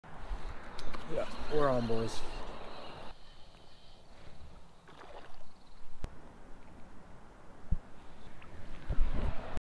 Beautiful Late Season Brown Trout
Beautiful brook trout caught in a small creek.